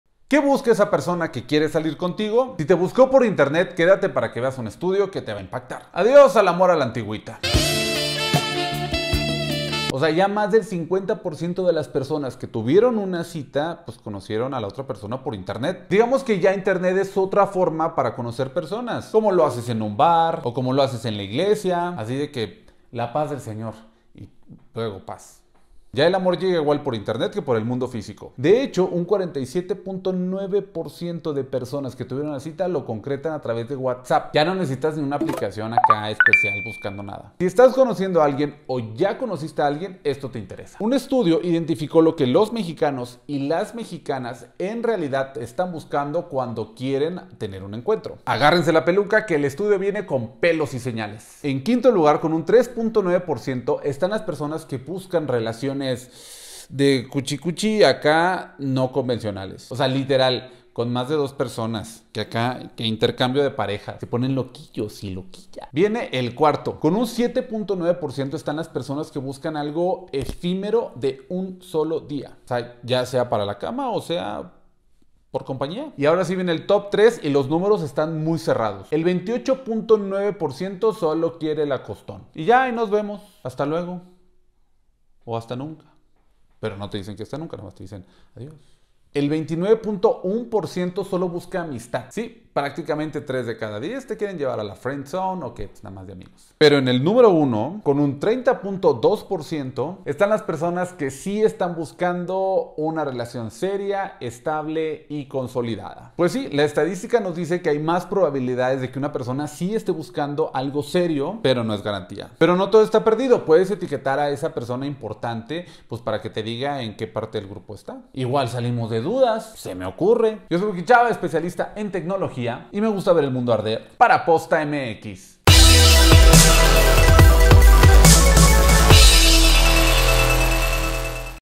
WikiChip | Amor o s3x0 esto es lo que buscan los mexicanos por Internet
¿Qué busca esa persona que quiere salir contigo?
Si te buscó por Internet quédate para que veas este estudio que te va a impactar

Adiós al amor a la antigüita, ya más del 50% de las personas que tuvieron un ligue pues lo hicieron por Internet…

Digamos que ya internet es otra forma de conocer el amor, como salir a conocer a un bar, o hasta en la iglesia. Que La Paz del señor y que se yo…

Ya, el amor llega igual por Internet que por el mundo físico. De hecho 47.9% utiliza WhatsApp para concretar las citas o el encuentro. Si estás conociendo a alguien o ya conociste a alguien esto te interesa: Un estudio identificó lo que mexicanos y mexicanas en realidad buscan cuando quieren tener una cita con alguien…

Agárrense la peluca, que el estudio viene con pelos y señales

En quinto lugar, con un 3.9% buscan relaciones sexuales no convencionales: literal mas de dos, que intercambio de parejas y se ponen mas loquillos y loquillas.

Viene el cuarto: 7.9% buscan compañía de un solo día sí algo ultra efímero, ya sea en la cama o compañía nada mas.

Ahora sí viene el top  3 y los números están muy cerrados:
Un 28.9% solo quiere el acostó y ya, nos vemos luego casi nunca.
El 29.1% solo quiere amistad… Sí, prácticamente 3 de cada 10 solo buscan llevarte a la friendzone.

Pero en número uno con el 30.2% están los que quieren una relación estable y consolidada.
Pues sí, la estadística nos dice que hay más probabilidades de que esa persona quiera algo serio, pero no hay garantía.
Pero no todo está perdido, puedes etiquetar a alguien importante para que te diga en qué grupo está. Igual salimos de dudas, se me ocurre.
Yo Soy Wikichava especialista en tecnología y me gusta ver el mundo arder…
Para POSTA MX